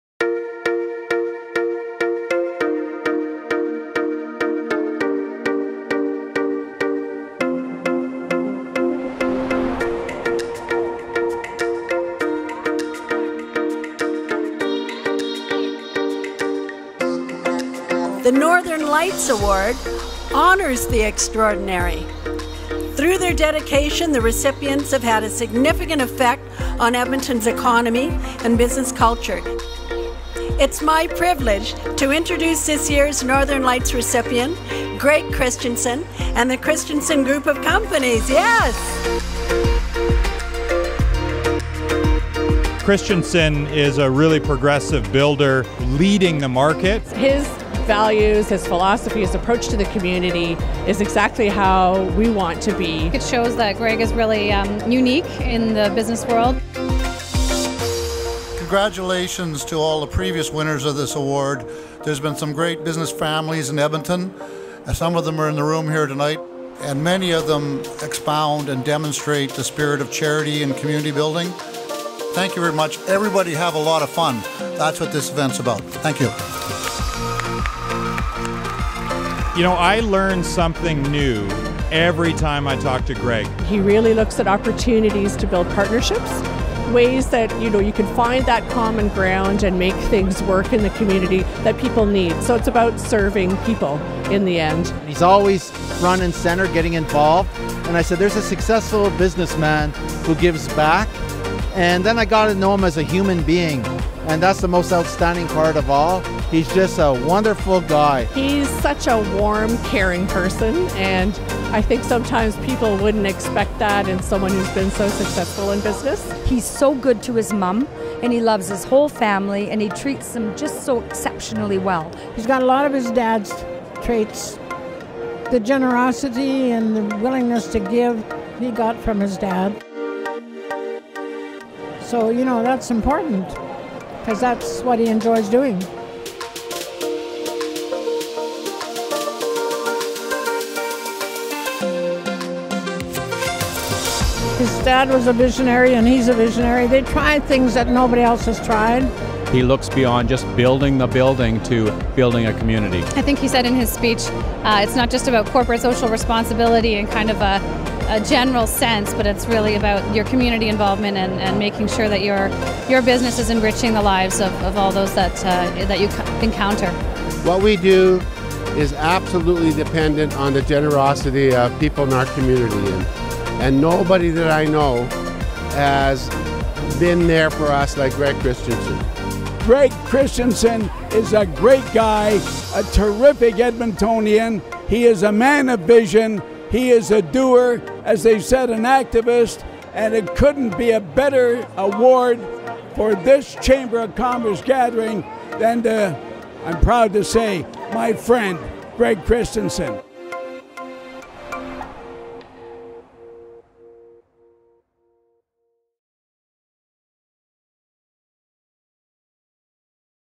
Edmonton Chamber of Commerce Northern Lights Award Recipient 2018 - Greg Christenson
The Christenson Group Of Companies CEO, Greg Christenson, was honoured for his charitable endeavours and contributions to our city by The Edmonton Chamber of Commerce.